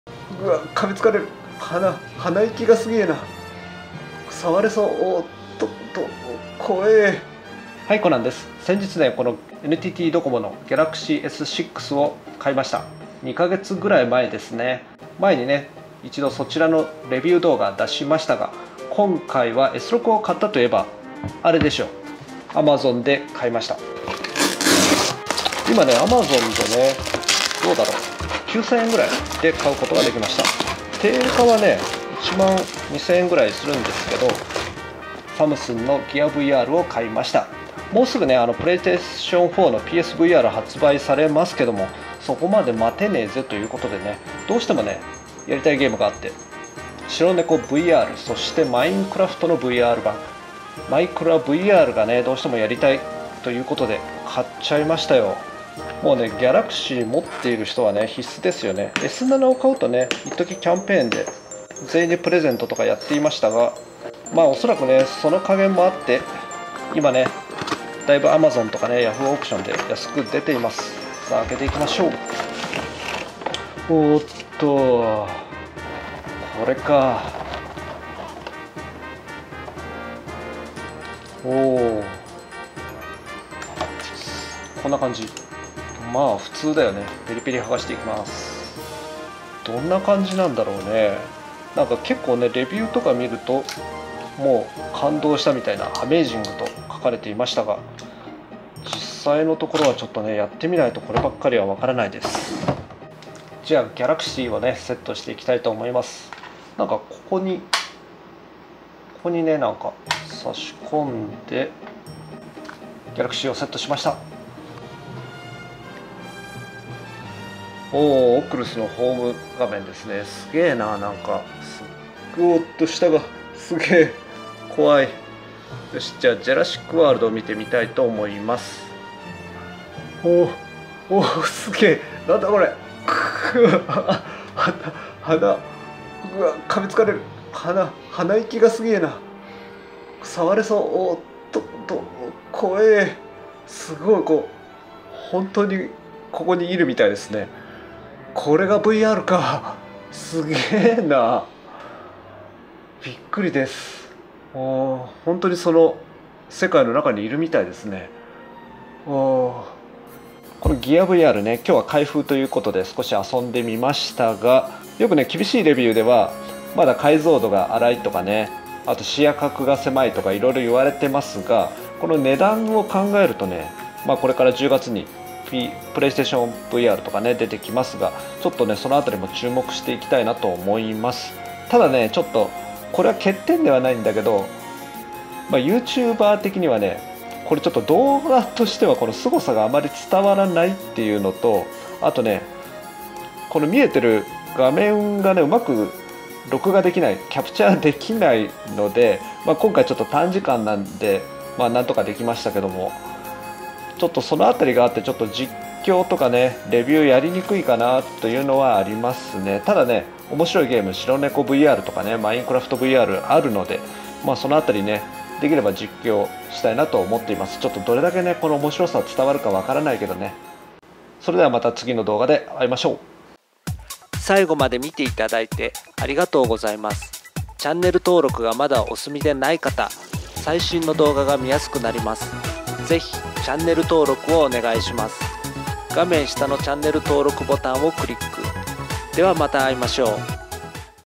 PSVRまで待てない！GearVRで360 3D映像、ゲームを体験
360度３D映像のVRはその場にいるような臨場感。ゲームも別次元だ。サムスンのGalaxyで楽しめるGearVR、本格的なVR体験がすごいです。
Here's my review of Samsung Gear VR with GalaxyS6.

Equipment
Mobile：Sumsung GalaxyS6 SC-05G(NTT DOCOMO)　Android6.0 Marshmallow
Maicrophone：SONY ECM-80U
Camera：CANON Ivis HF G20